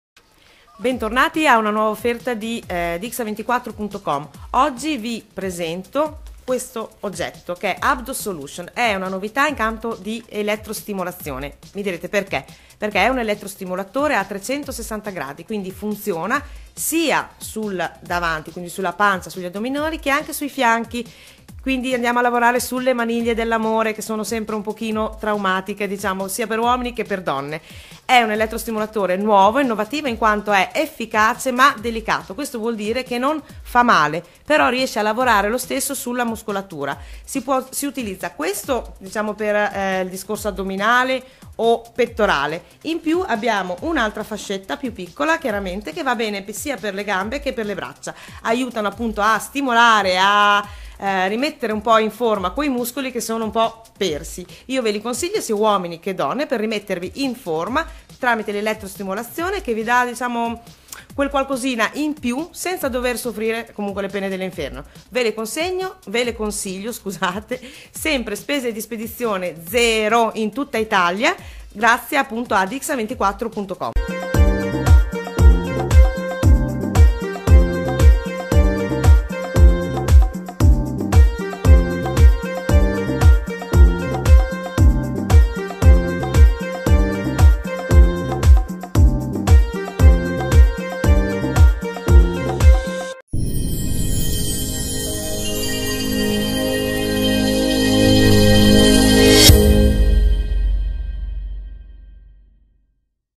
ABDO SOLUTION – ELETTROSTIMOLATORE CON MICROPROCESSORE EEM + BORSA | DXA24.COM
Puoi acquistare il prodotto qui:
Spese di spedizione 6,90€ - Pagamento alla consegna

Direttamente su Dxa 24, un nuovo favoloso alleato della tua forma fisica che ti aiuterà a bruciare il grasso presente nel tuo corpo senza sforzo!
Abdo Solution è una fascia top ab con microprocessore che utilizza l’elettrostimolazione eem. Si tratta di una tecnologia sviluppata da fisioterapisti, per aiutarti a mantenere il tuo tono muscolare e sviluppare i tuoi addominali. Funziona generando delle contrazioni attive nei tuoi muscoli e ti permette di svolgere qualsiasi altra attività mentre lo usi.
Abdo solution arriva direttamente a casa tua completo di accessori e borsa da trasporto!
La palestra non è più l’unica opzione per tenerti in forma! C’è Abdo solution!